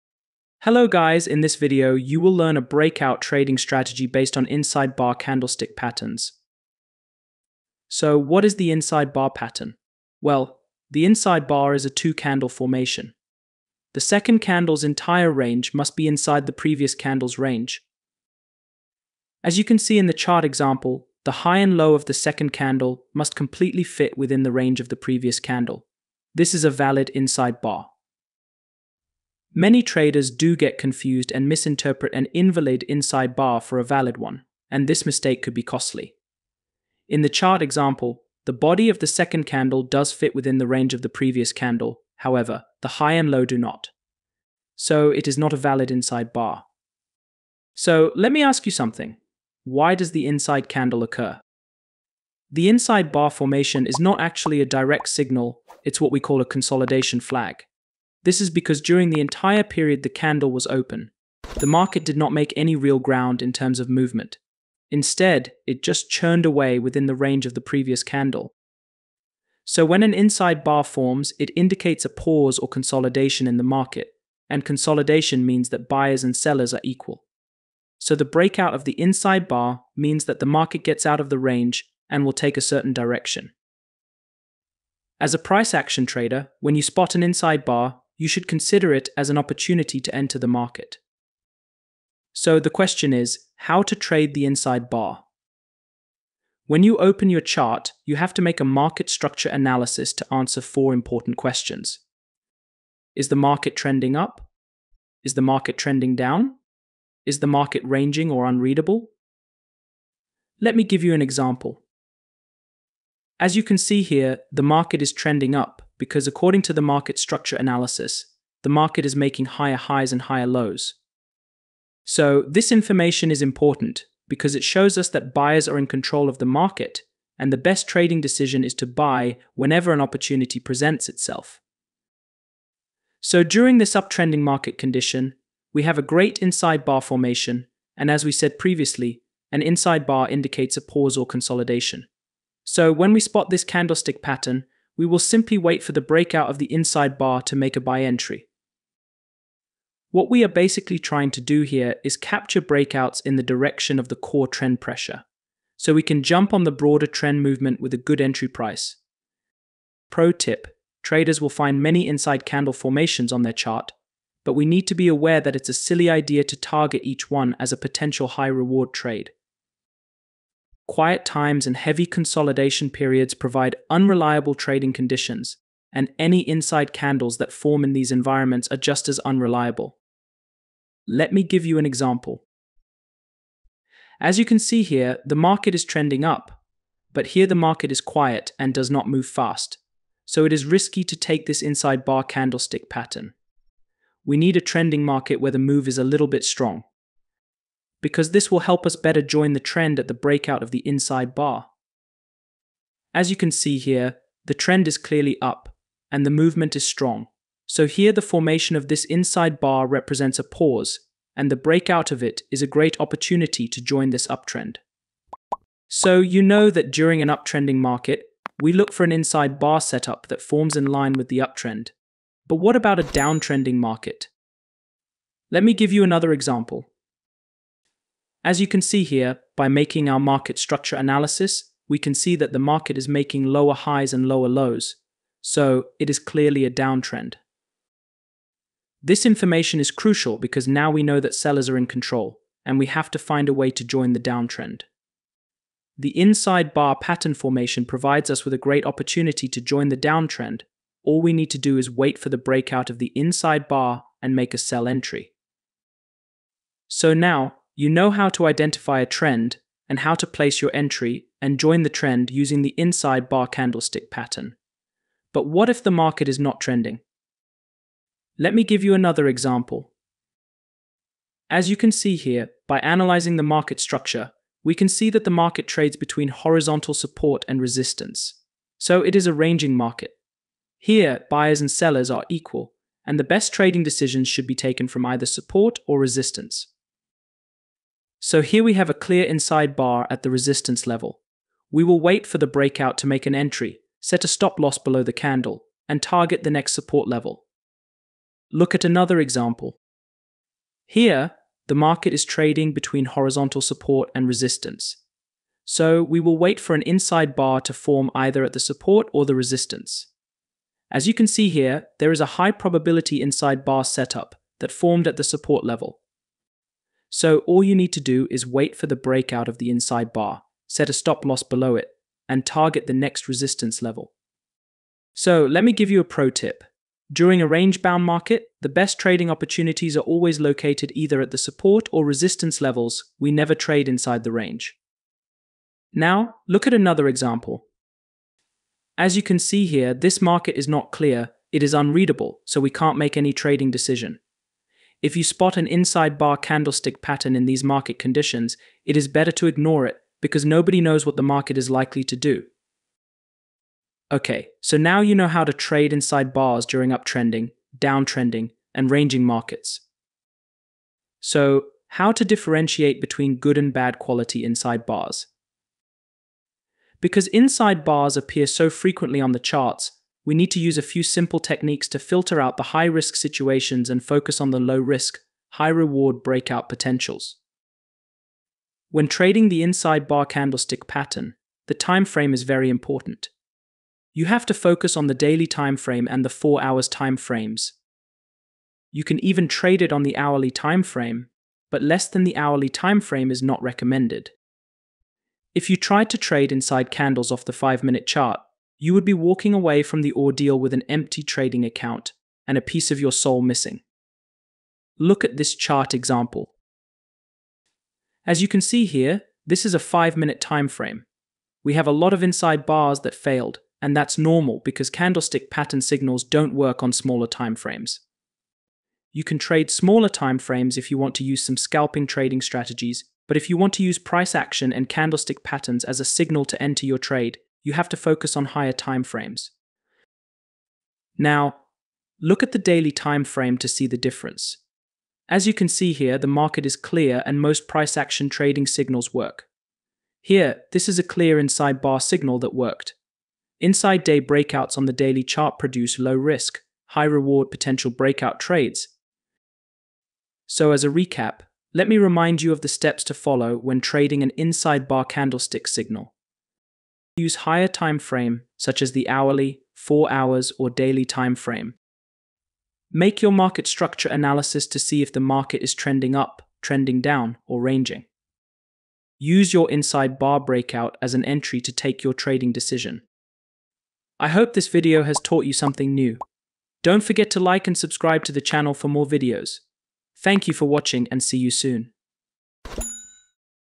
The Inside Bar Breakout Trading Strategy You Have Been Waiting For
In this video, you will learn a breakout trading strategy based on inside bar candlestick patterns.

So, what is the Inside Bar pattern? Well, the Inside Bar is a two-candle formation. The name says it all, really, because the second candle’s entire range must be inside the previous candle’s range.
So when an inside bar forms, it indicates a pause or consolidation in the market, and consolidation means that buyers and sellers are equal. So the breakout of the inside bar means that the market gets out of the range and will take a certain direction.
As a price action trader, when you spot an inside bar, you should consider it as an opportunity to enter the market.
So, the question is, how to trade the inside bar?
When you open your chart, you have to make a market structure analysis to answer four important questions:
⁃ Is the market trending up?
⁃ Is the market trending down?
⁃ Is the market ranging?
⁃ Is the market unreadable?
When trading the inside bar candlestick pattern, the time frame is very important. You have to focus on the Daily Time frame and the H4 time frames. You can even trade it on the hourly time frame, but less than the hourly time frame is not recommended.

If you tried to trade Inside Candles off the 5-minute chart, you would be walking away from the ordeal with an empty trading account and a piece of your soul missing.